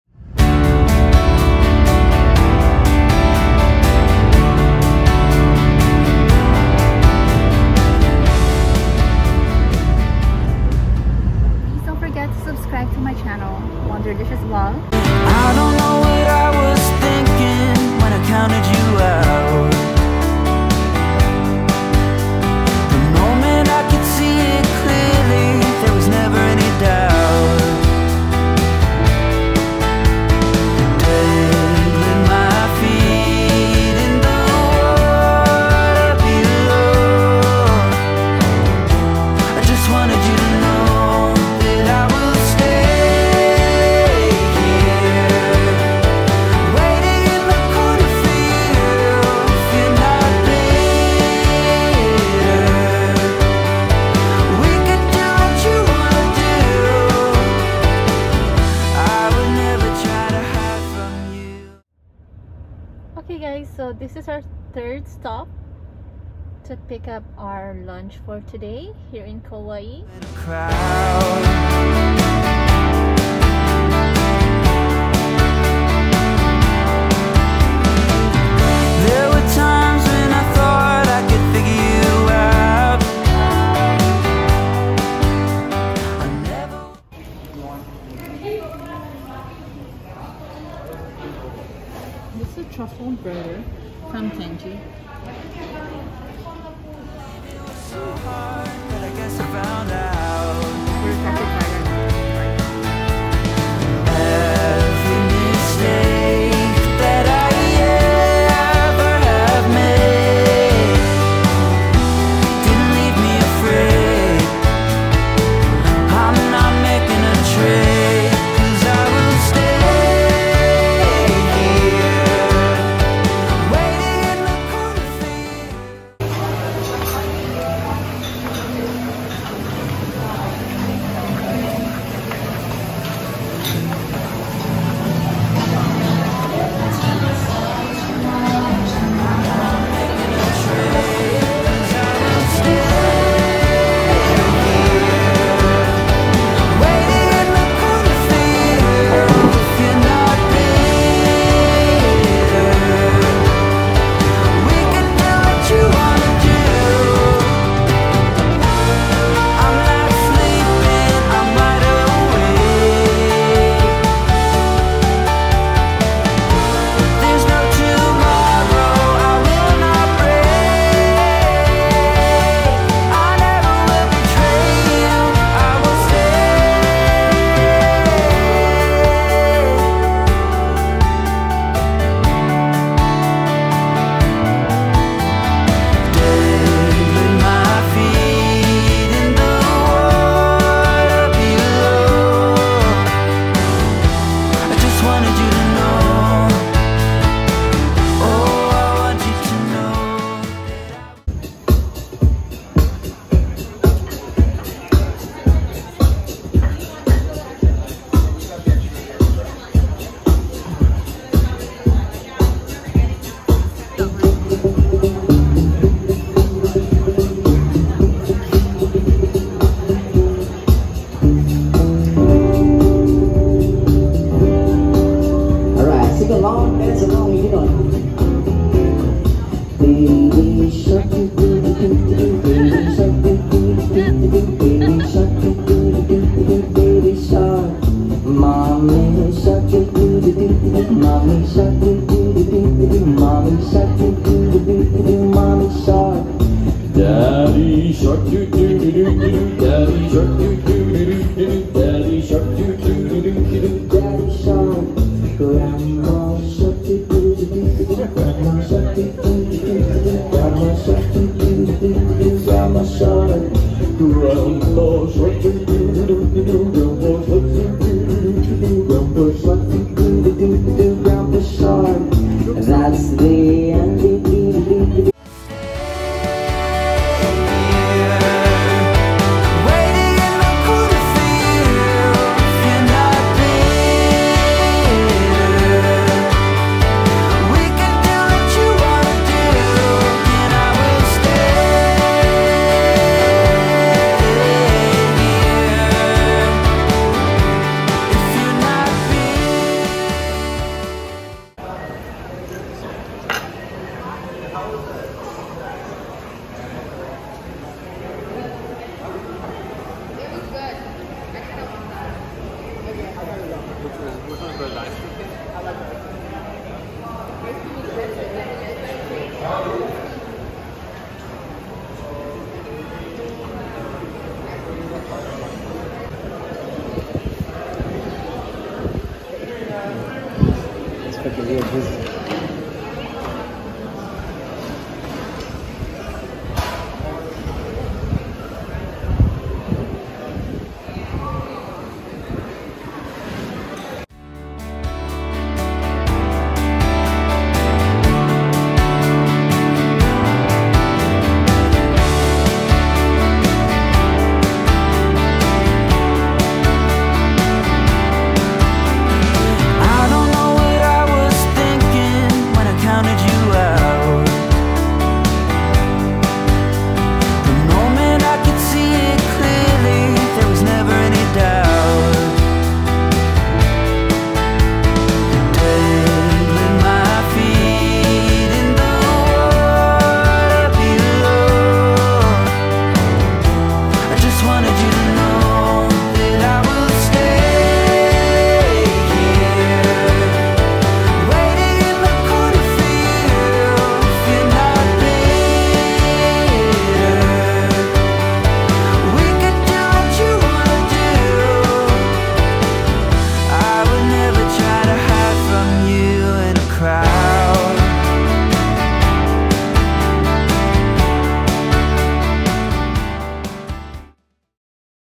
Where Locals Eat in Kauai
Shave Ice and Manju from Luke Zane’s First Birthday Party
Lychee from Jen

Weekend Trip to Kauai Part 4 is coming soon…